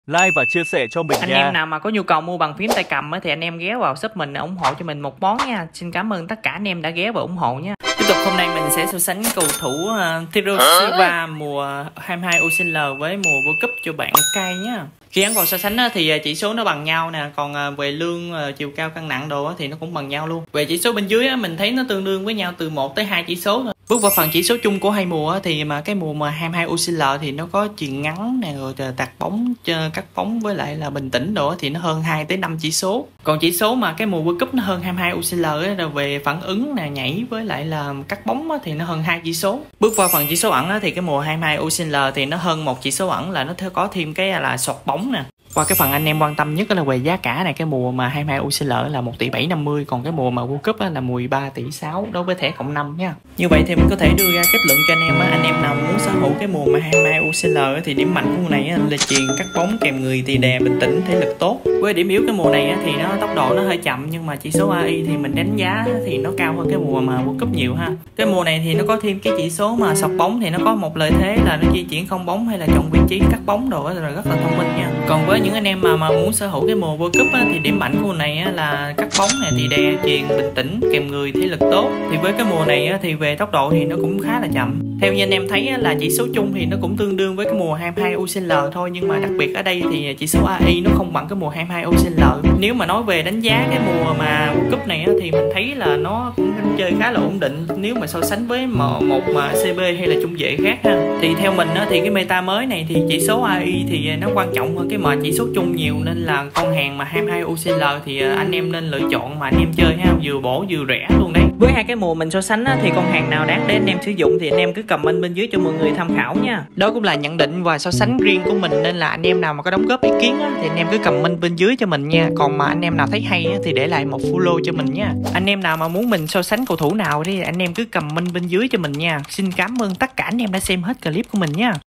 SO - SÁNH || Thiago Silva Mùa 22UCL vs Mùa WC22 || Xem Mùa Nào Đáng Để AE Mình Trải Nghiệm.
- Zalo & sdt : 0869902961